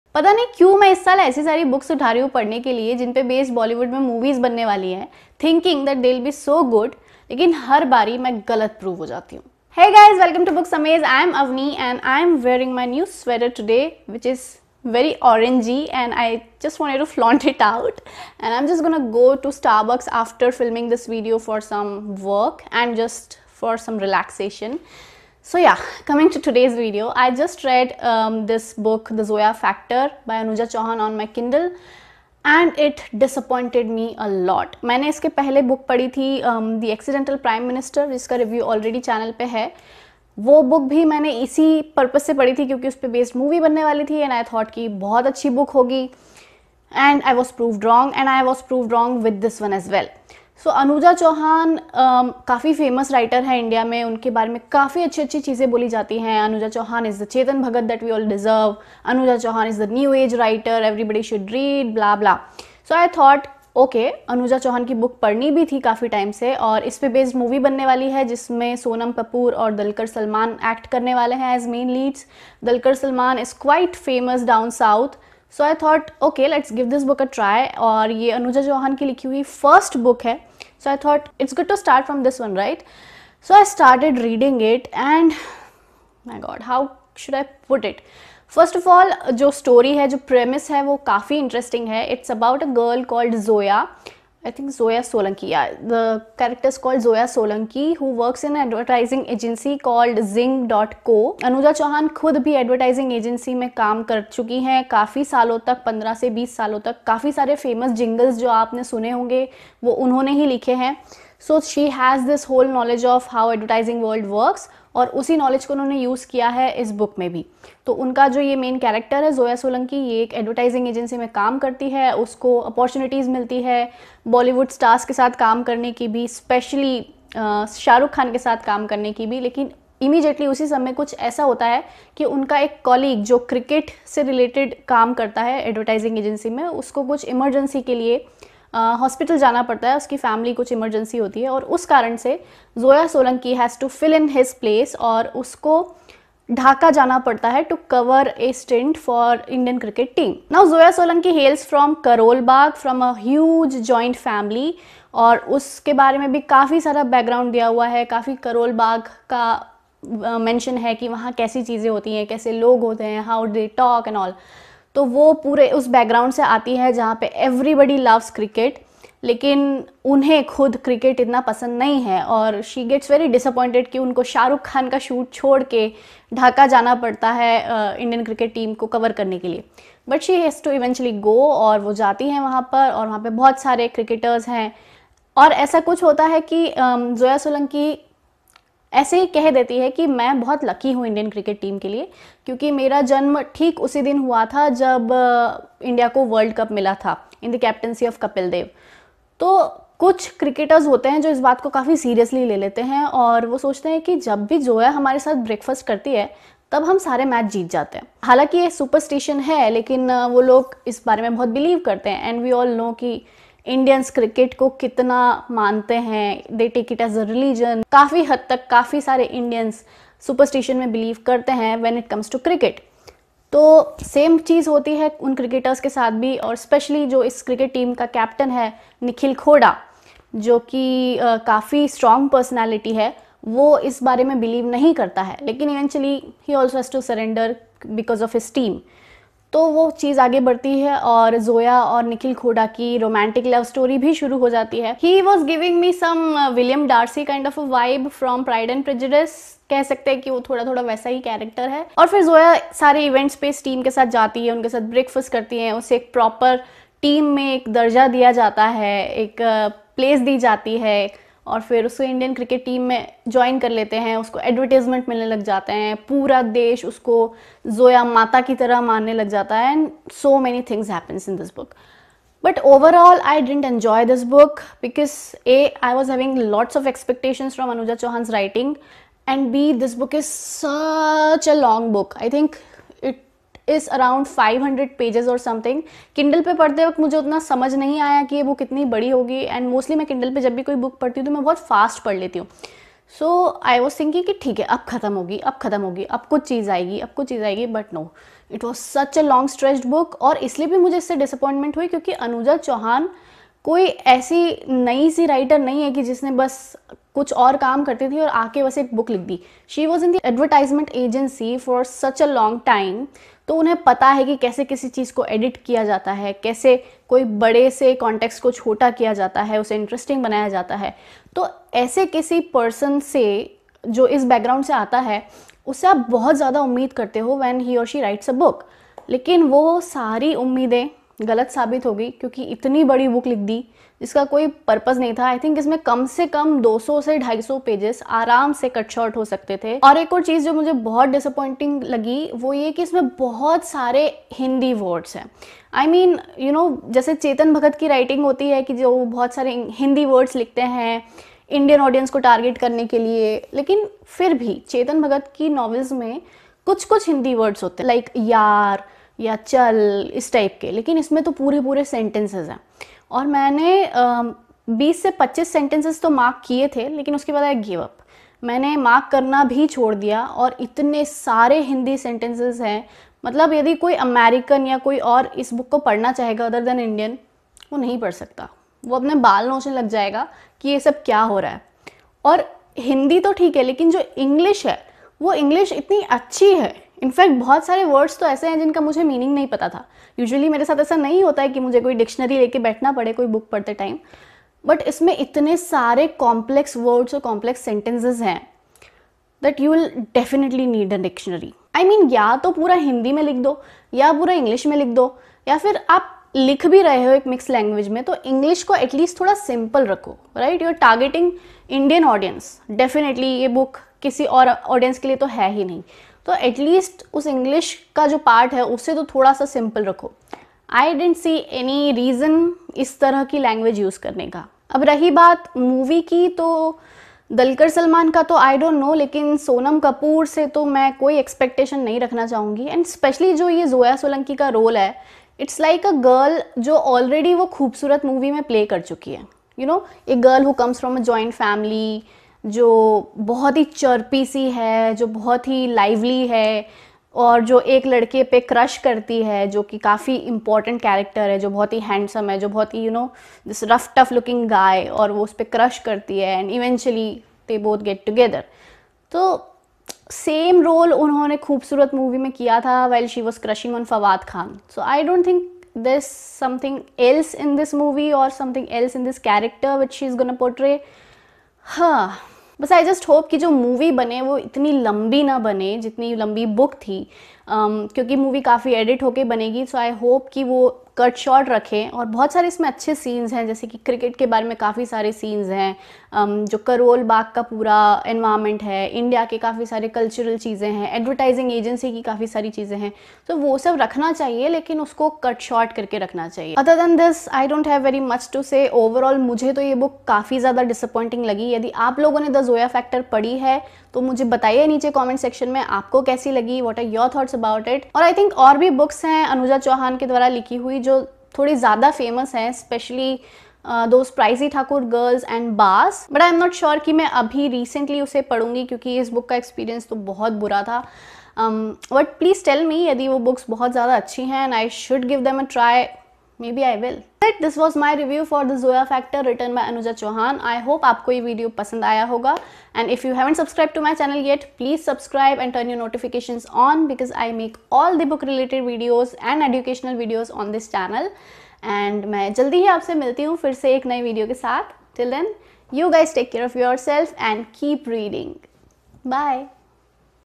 The Zoya Factor By Anuja Chauhan Rant Review In Hinglish
A Hinglish Video for the book - The Zoya Factor which I recently read.